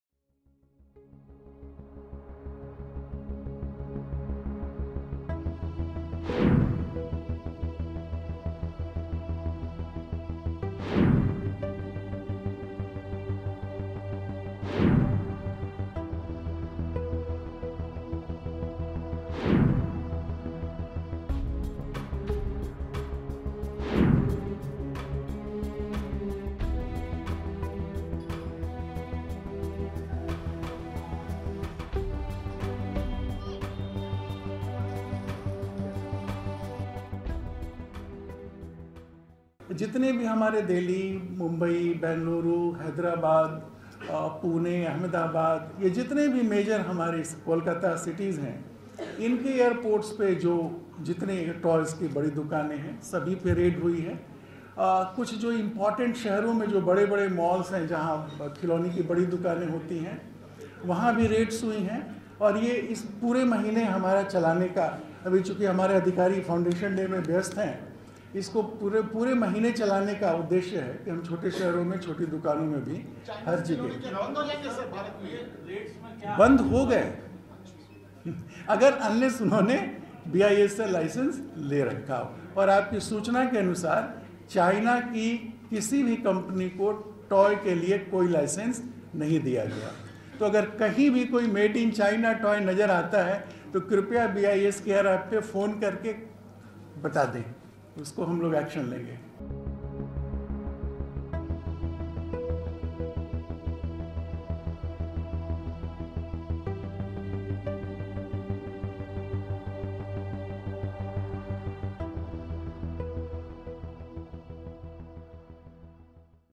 Substandard Toys Made In China Seized In Countrywide Raids
The Bureau of Indian Standards (BIS) has conducted countrywide raids at toy outlets at Airports and Malls and found substandard toys made in China. The raids will continue this month to check the substandard toys.   BIS Director General Pramod Kumar Tiwari said, "Countrywide raids have been conducted at toy outlets at Airports and Malls and found substandard toys made in China. The raids will continue this month to check the substandard toys. We have not given a license to any Chinese company to make toys in India. People should inform BIS if they found Made in China toys in the country."